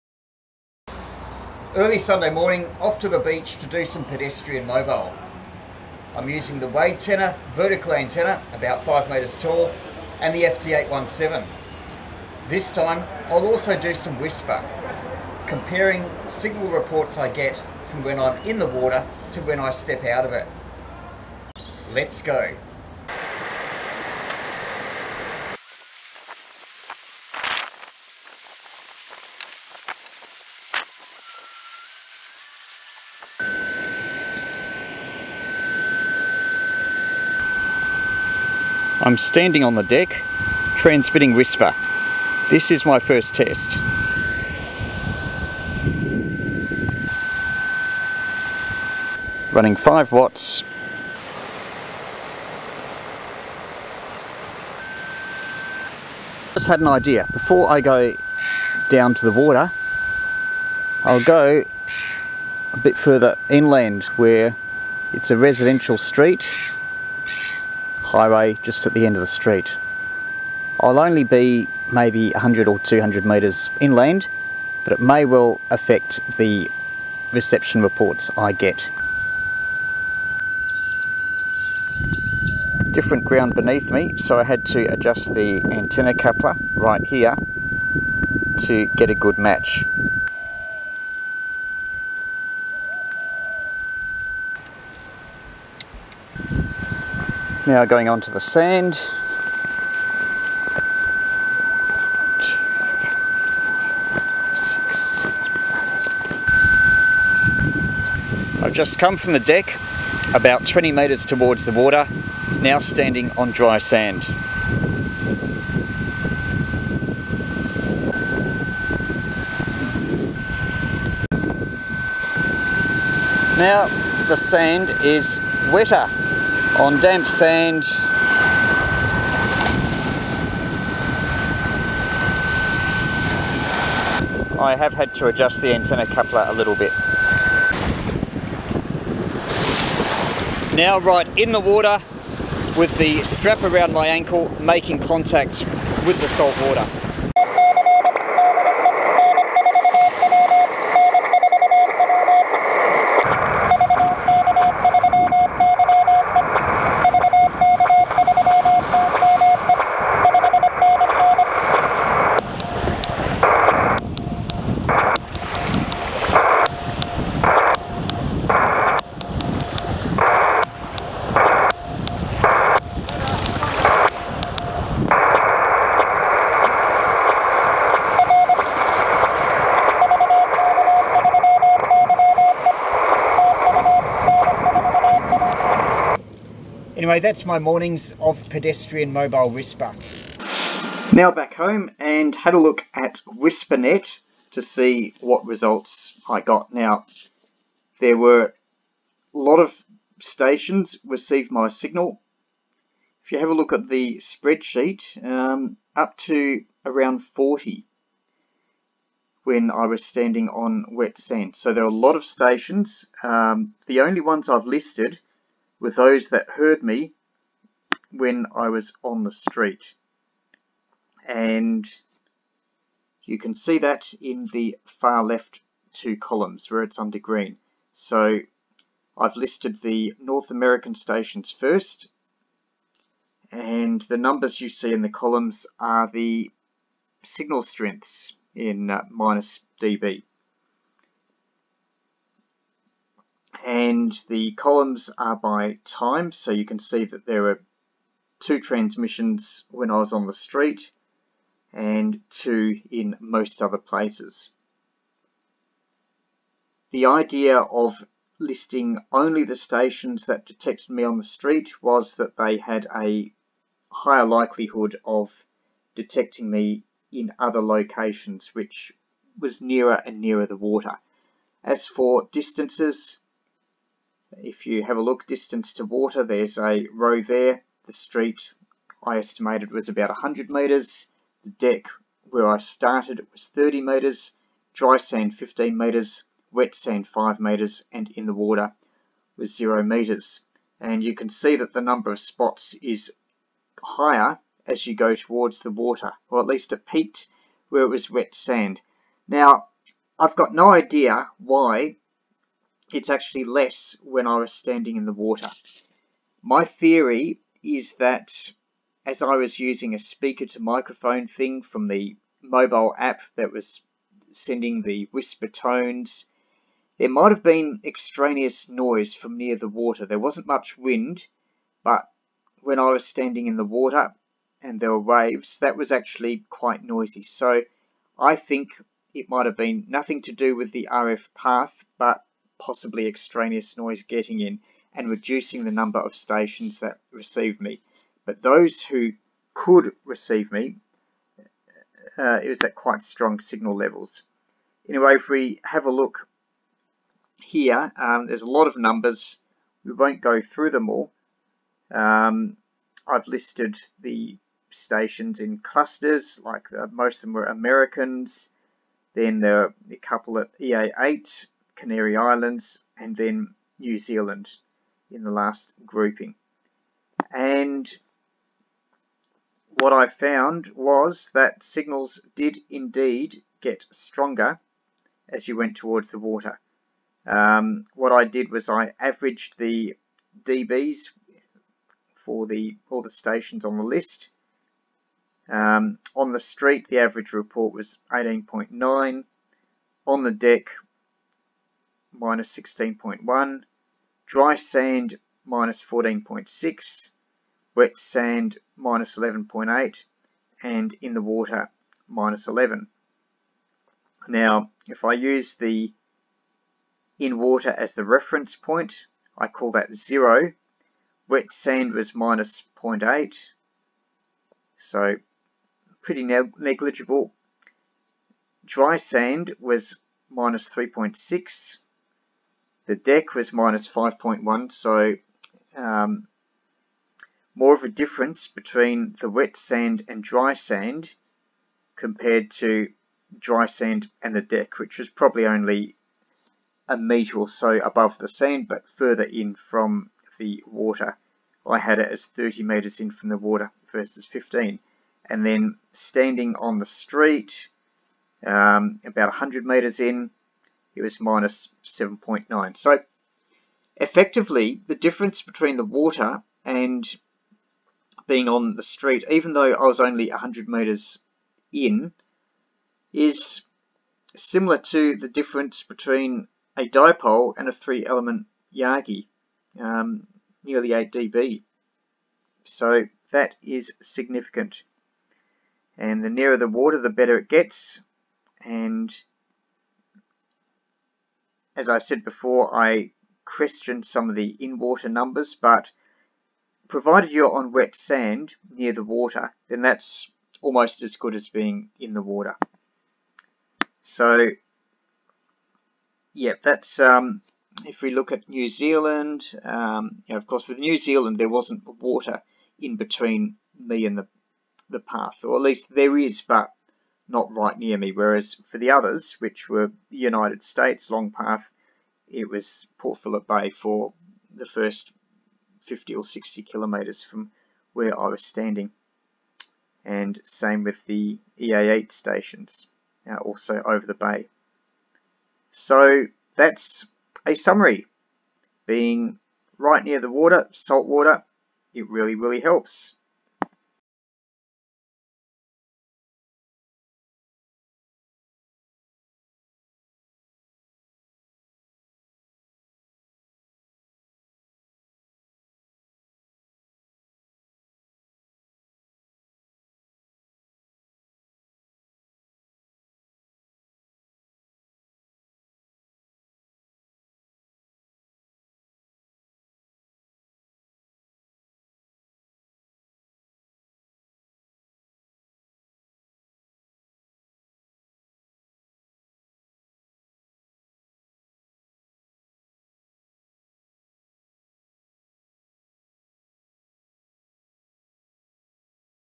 Salt water: How many dB better?
Pedestrian mobile WSPR tests on 14 MHz. Walking in and out of salt water.

Chapters
0:00 Intro
0:23 WSPR tx various locations
2:27 Ped mobile CW contact N4W
3:06 Results and discussion
8:53 My book